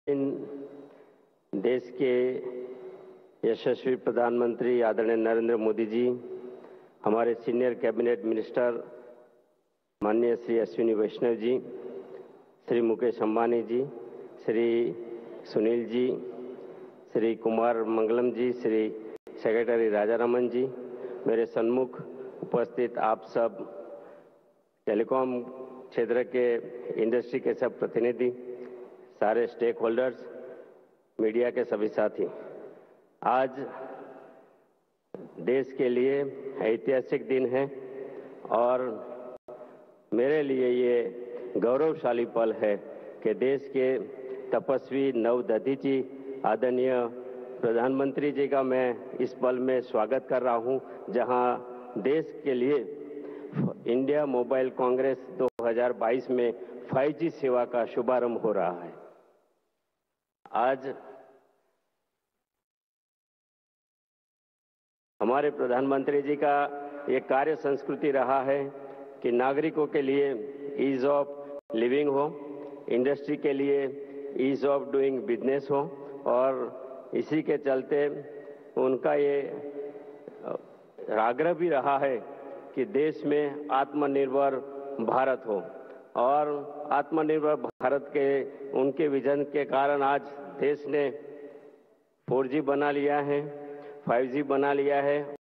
Launch Of 5G Services | '5G Services To Rollout Across India Within 2 Years': Dev Singh Chauhan
According to the latest updates on the 5G service launch in India, Dev Singh Chauhan said that it is a historic day for India and I am feeling honored to welcome PM Modi to the 5g launch event. He also said India will be covered with a 5G network within two years. PM Modi launched 5g services today at around 10 am at the India Mobile Congress 2022 in New Delhi where the Prime Minister will also be visiting the exhibition to witness the demonstration of the use of the 5g technology in multiple areas. According to the sources, 5g technology will provide seamless coverage, high data rate, low latency, and highly reliable communication. It will also increase energy efficiencies, spectrum efficiencies, and network efficiencies.